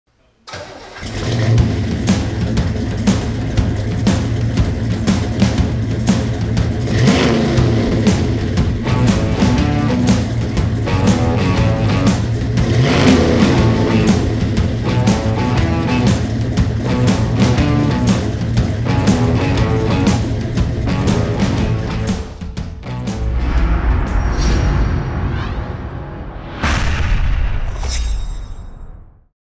1970 Chevelle SS454 LS6 4-Speed FOR SALE
Real deal 1970 Chevelle SS454 LS6 4-speed for sale! Ready for a new owner.